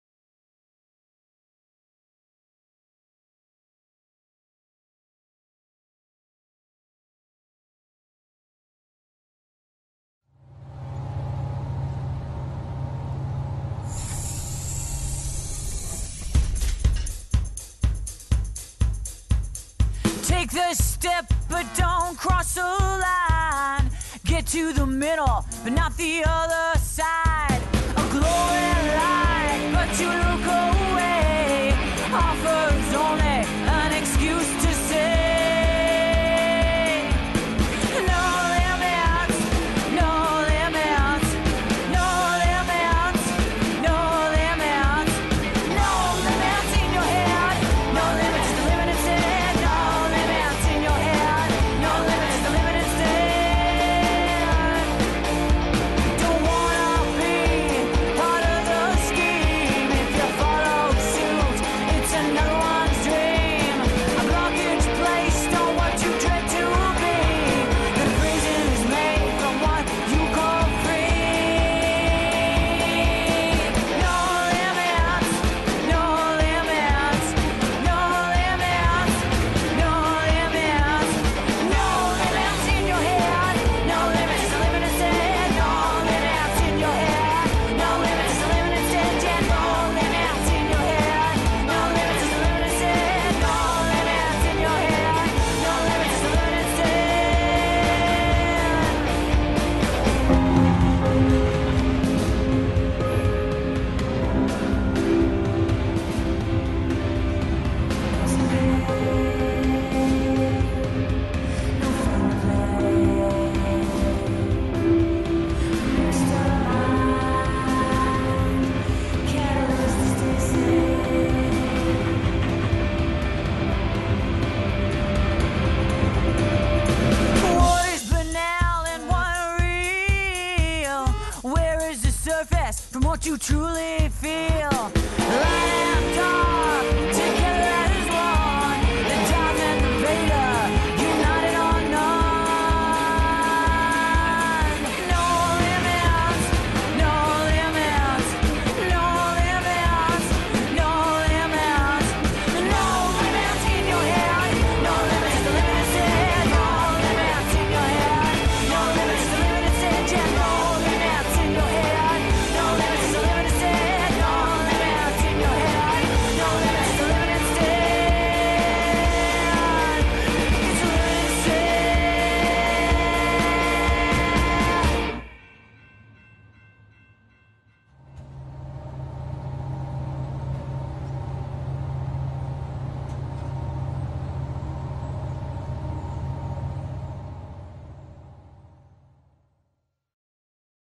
Kris Reichert: da MTV a The Jains
In pochi non si ricorderanno delle due biondissime e quasi identiche Vj di MTV Kris and Kris. Una delle due, Kris Reichert, ora ha intrapreso la strada della musica nel duo femminile delle The Jains. Scoprila in questa nuova veste rock e poi lascia un tuo commento!